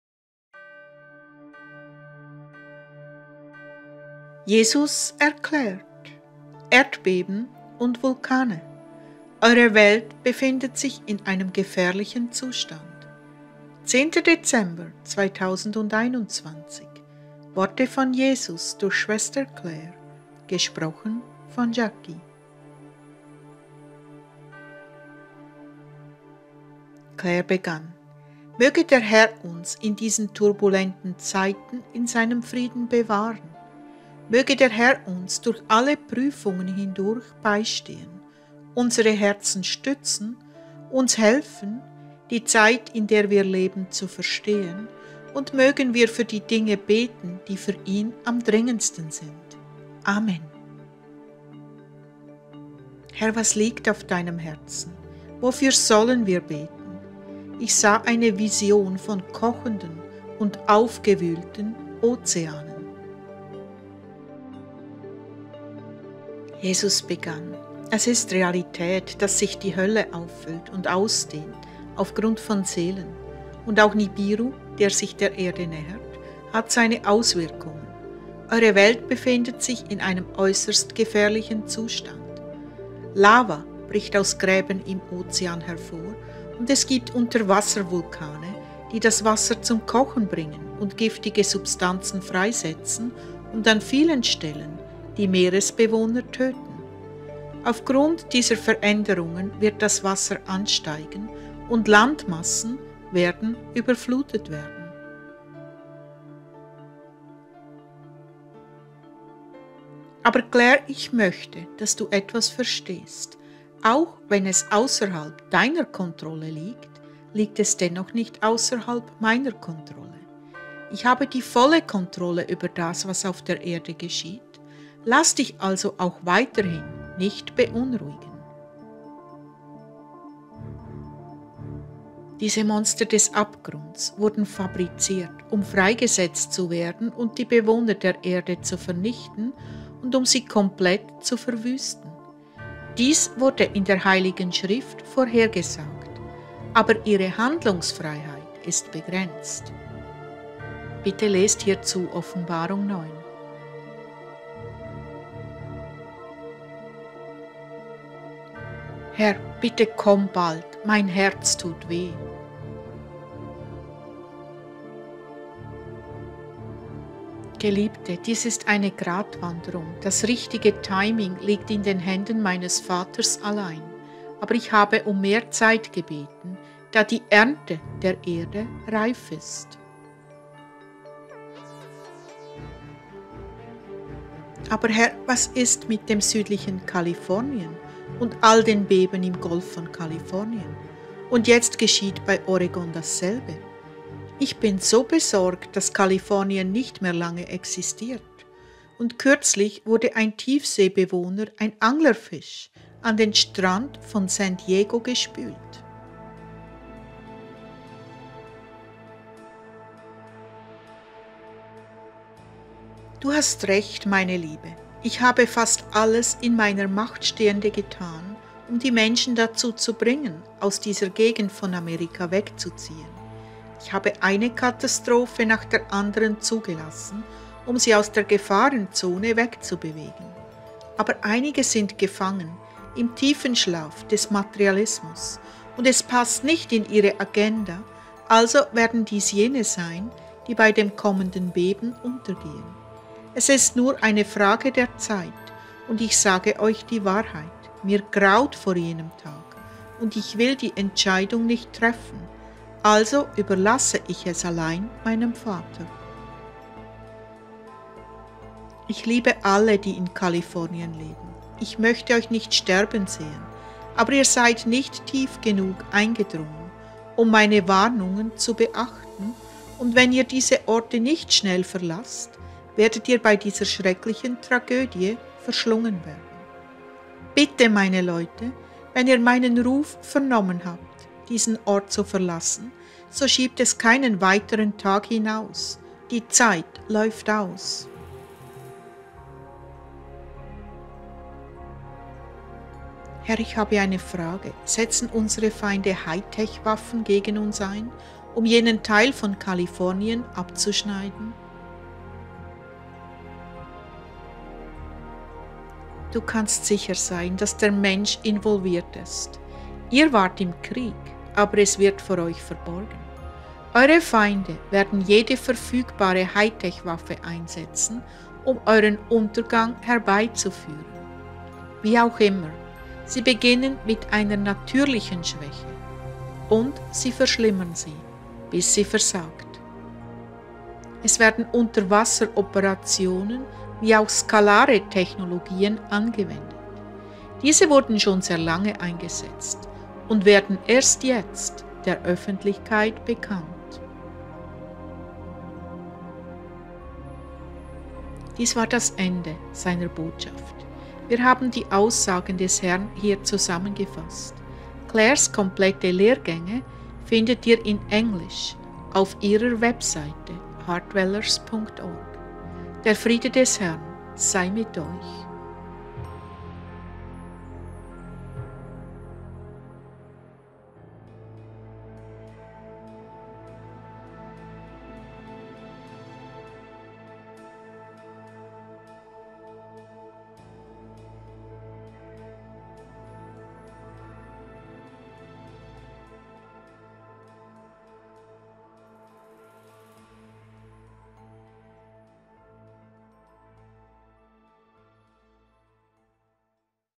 Rhema 23.03.2022 ❤️ Erdbeben & Vulkane... Verlasst Kalifornien so schnell wie möglich !
Erdbeben & Vulkane... Eure Welt befindet sich in einem gefährlichen Zustand

10. Dezember 2021 - Worte von Jesus durch Schwester Clare

(Clare) Möge der Herr uns in diesen turbulenten Zeiten in Seinem Frieden bewahren. Möge der Herr uns durch alle Prüfungen hindurch beistehen, unsere Herzen stützen, uns helfen, die Zeit, in der wir leben, zu verstehen und mögen wir für die Dinge beten, die für Ihn am dringendsten sind... Amen.

'Herr, was liegt auf Deinem Herzen? Wofür sollen wir beten? Ich sah eine Vision von kochenden und aufgewühlten Ozeanen.'

(Jesus) "Es ist Realität, dass sich die Hölle auffüllt und ausdehnt aufgrund von Seelen und auch Nibiru, der sich der Erde nähert, hat seine Auswirkungen. Eure Welt befindet sich in einem äusserst gefährlichen Zustand. Lava bricht aus Gräben im Ozean hervor und es gibt Unterwasservulkane, die das Wasser zum Kochen bringen und giftige Substanzen freisetzen und an vielen Stellen die Meeresbewohner töten. Aufgrund dieser Veränderungen wird das Wasser ansteigen und Landmassen werden überflutet werden.

"Aber Clare, Ich möchte, dass du etwas verstehst... Auch wenn es ausserhalb deiner Kontrolle liegt, liegt es dennoch nicht ausserhalb Meiner Kontrolle. Ich habe die volle Kontrolle über das, was auf der Erde geschieht. Lass dich also auch weiterhin nicht beunruhigen.

"Diese Monster des Abgrunds wurden fabriziert, um freigesetzt zu werden und die Bewohner der Erde zu vernichten und um sie komplett zu verwüsten. Dies wurde in der Heiligen Schrift vorhergesagt, aber ihre Handlungsfreiheit ist begrenzt." (Bitte lest hierzu Offenbarung 9)

(Clare) 'Herr, bitte komm bald, mein Herz tut weh.'

(Jesus) "Geliebte, dies ist eine Gratwanderung, das richtige Timing liegt in den Händen Meines Vaters allein, aber Ich habe um mehr Zeit gebeten, da die Ernte der Erde reif ist."

(Clare) 'Aber Herr, was ist mit dem südlichen Kalifornien und all den Beben im Golf von Kalifornien... und jetzt geschieht bei Oregon dasselbe... Ich bin so besorgt, dass Kalifornien nicht mehr lange existiert. Und kürzlich wurde ein Tiefseebewohner, ein Anglerfisch, an den Strand von San Diego gespült. '

(Jesus) "Du hast Recht. Meine Liebe, Ich habe fast alles in Meiner Macht stehende getan, um die Menschen dazu zu bringen, aus dieser Gegend von Amerika wegzuziehen. Ich habe eine Katastrophe nach der Anderen zugelassen, um sie aus der Gefahrenzone wegzubewegen. Aber Einige sind gefangen im tiefen Schlaf des Materialismus und es passt nicht in ihre Agenda, also werden dies Jene sein, die bei dem kommenden Beben untergehen. Es ist nur eine Frage der Zeit und Ich sage euch die Wahrheit, Mir graut vor jenem Tag und Ich will die Entscheidung nicht treffen, also überlasse Ich es allein Meinem Vater.

"Ich liebe Alle, die in Kalifornien leben. Ich möchte euch nicht sterben sehen, aber ihr seid nicht tief genug eingedrungen, um Meine Warnungen zu beachten und wenn ihr diese Orte nicht schnell verlasst, werdet ihr bei dieser schrecklichen Tragödie verschlungen werden. Bitte, Meine Leute, wenn ihr Meinen Ruf vernommen habt, diesen Ort zu verlassen, so schiebt es keinen weiteren Tag hinaus. Die Zeit läuft aus."

(Clare) 'Herr, ich habe eine Frage... Setzen unsere Feinde Hightech-Waffen gegen uns ein, um jenen Teil von Kalifornien abzuschneiden?'

(Jesus) "Du kannst sicher sein, dass der Mensch involviert ist. Ihr wart im Krieg, aber es wird vor euch verborgen. Eure Feinde werden jede verfügbare High-Tech-Waffe einsetzen, um euren Untergang herbeizuführen. Wie auch immer, sie beginnen mit einer natürlichen Schwäche und sie verschlimmern sie, bis sie versagt. Es werden Unterwasser-Operationen wie auch skalare Technologien angewendet. Diese wurden schon sehr lange eingesetzt und werden erst jetzt der Öffentlichkeit bekannt."

(Jackie) Dies war das Ende Seiner Botschaft. Wir haben die Aussagen des Herrn hier zusammengefasst. Clare's komplette Lehrgänge findet ihr in englisch...